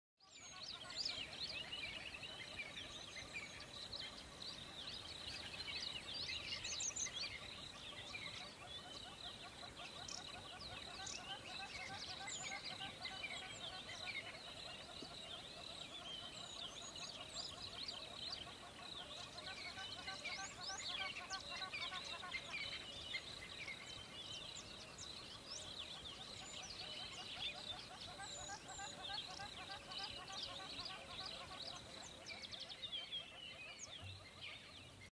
Black-crowned Night Heron stalking/fishing at a coastal wetland
Black-crowned Night Heron stalking/fishing at a coastal wetland in Southern California